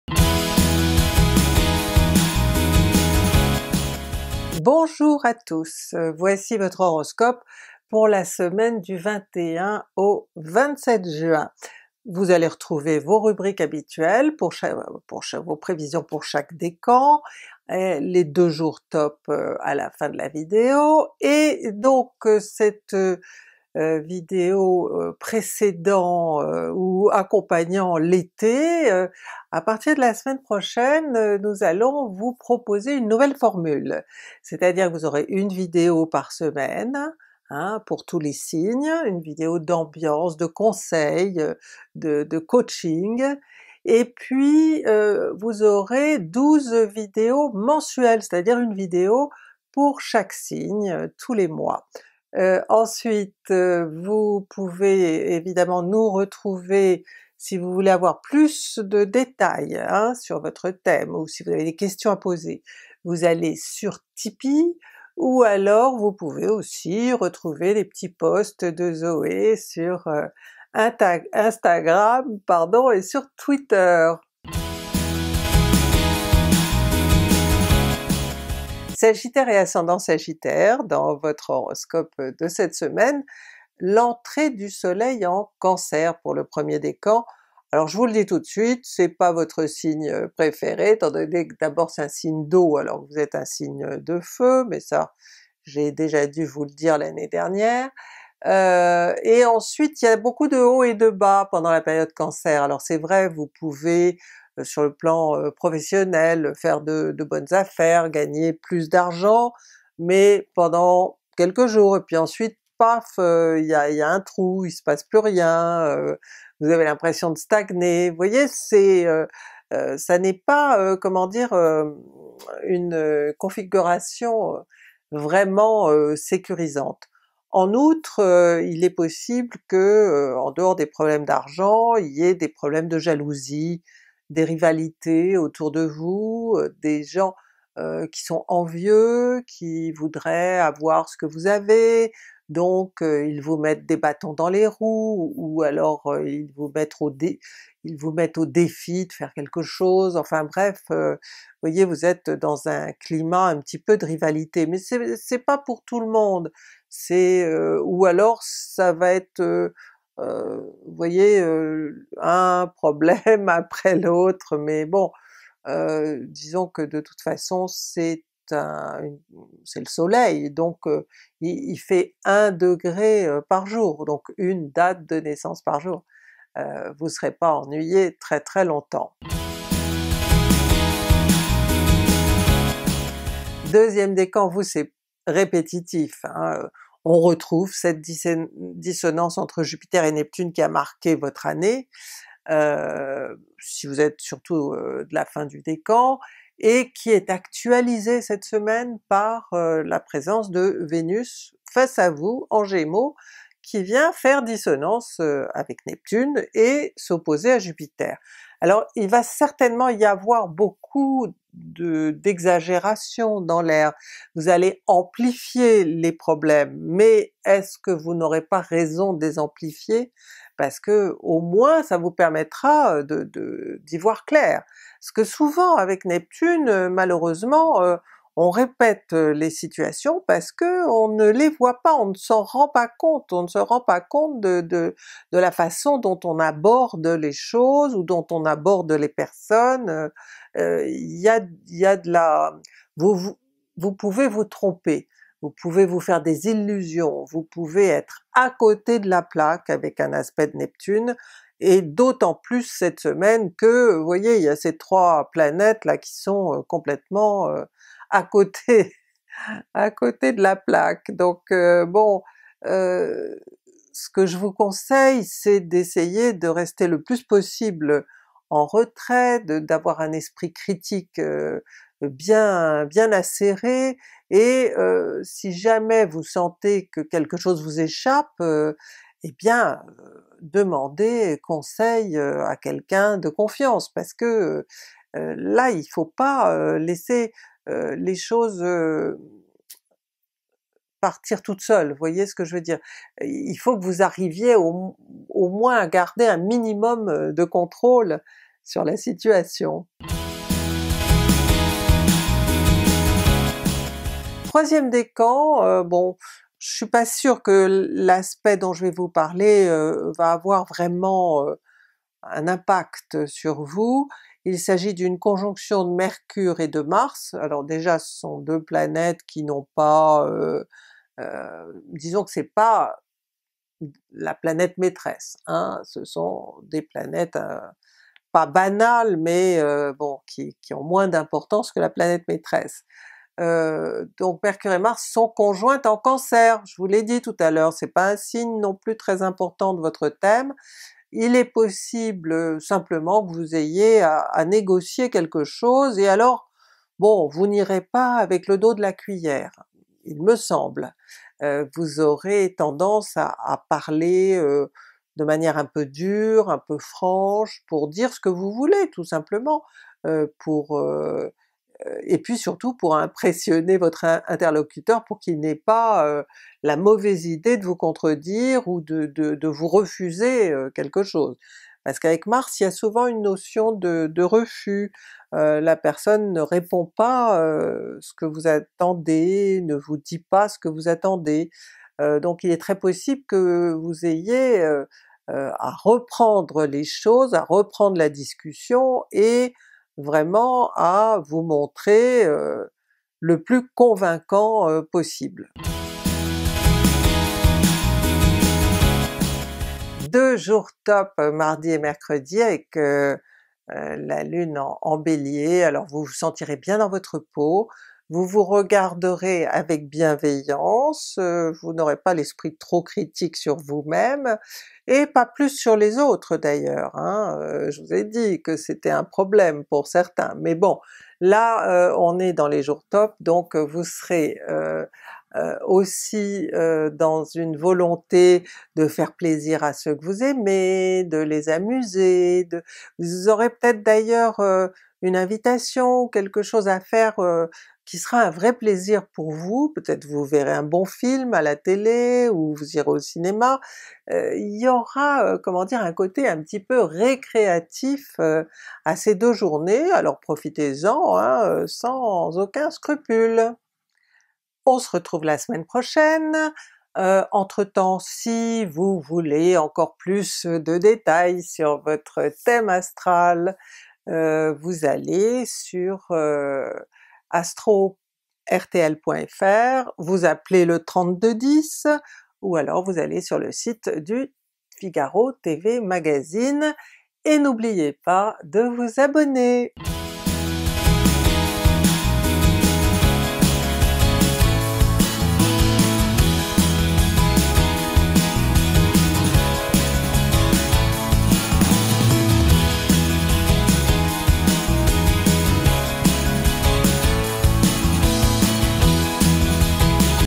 Horoscope Sagittaire ♐️ du 21 au 27 juin 2019 ✨
Sagittaire ♐️, quel est votre HOROSCOPE pour la SEMAINE du 21 au 27 juin 2019? Allez-vous passer une bonne semaine? Quel sera votre JOUR TOP? Comment l'ASTROLOGIE peut vous guider?

Je suis Christine HAAS, ASTRO-PSYCHOLOGUE (pour RTL et le TV Magazine). Découvrez votre HOROSCOPE pour la semaine du 21 au 27 juin 2019 si vous êtes Sagittaire ♐️ ou ASCENDANT Sagittaire ♐️.
Je vous conseille de regarder l'HOROSCOPE de votre signe et celui de votre ASCENDANT pour plus de précision. Et je vous rappelle que ces prévisions sont générales car je ne dispose pas de votre thème de naissance. Elles sont donc à prendre avec modération.